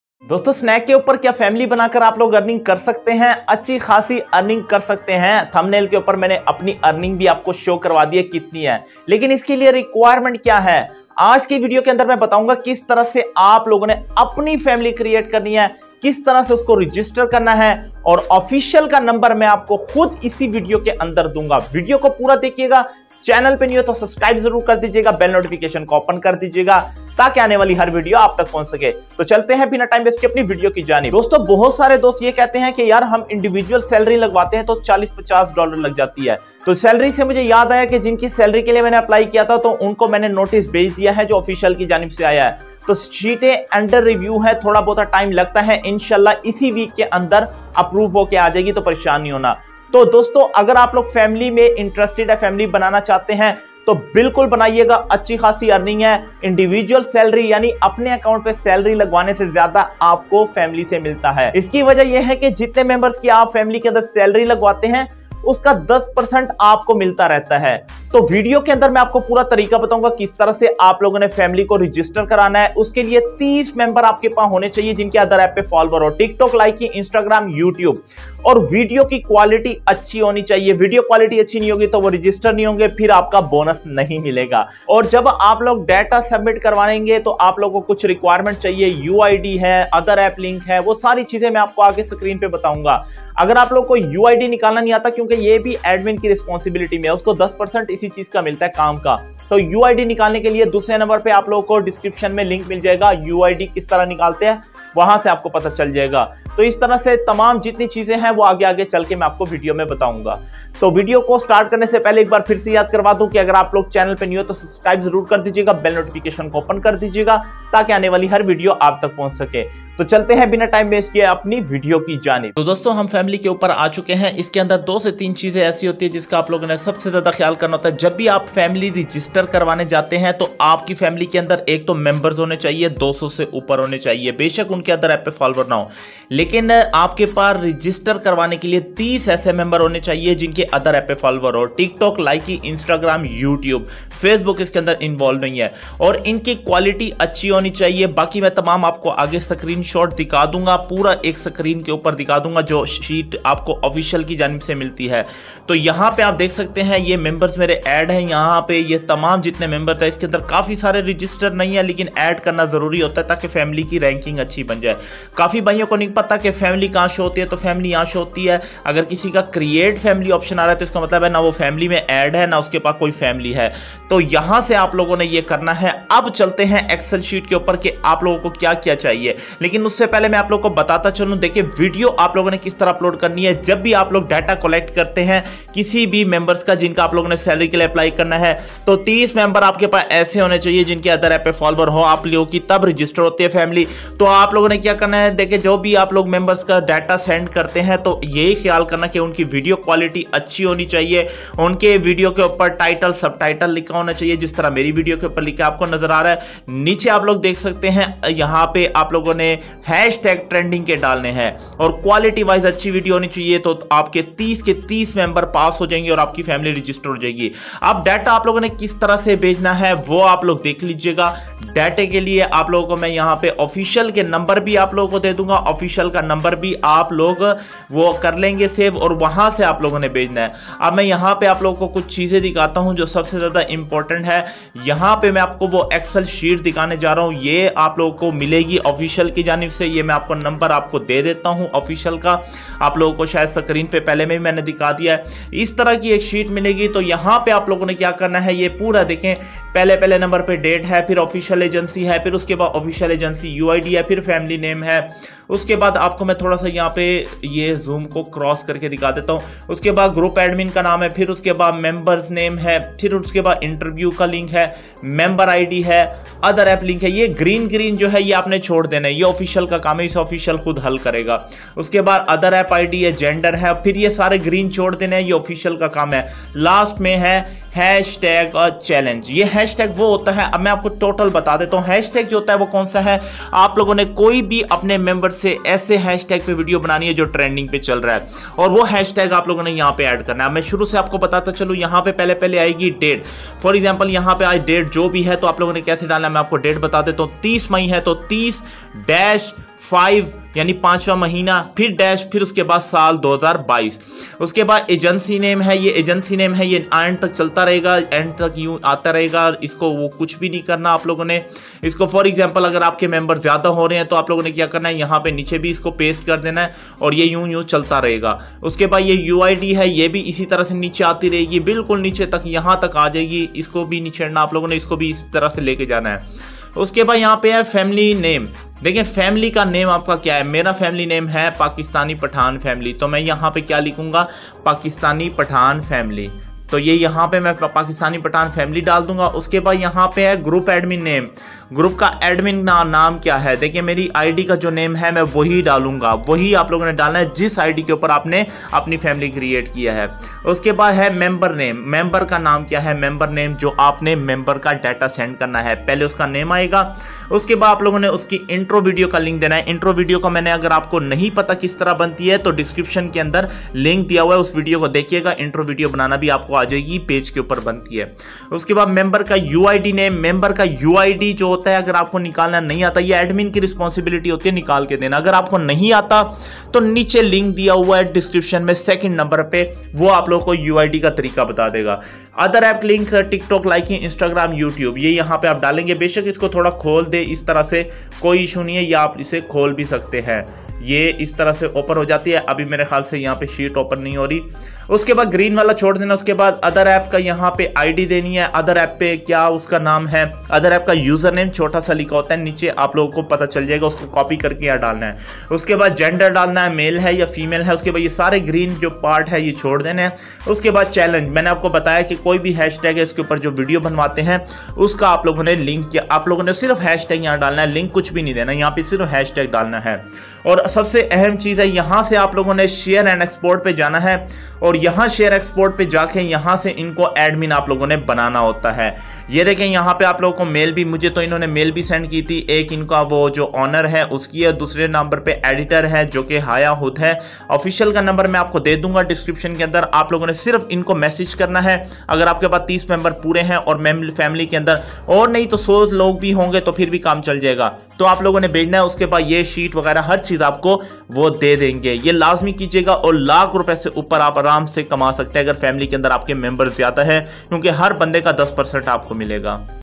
Snack video app par family banaye aur monthly 1 lakh Rupay kamaye |Snack family complete detail
Assalamualaikum! in this video I have explained about snack video and Snack video family earning. On snack video you can also create your own family and earn a lot of money by registration of snack video account.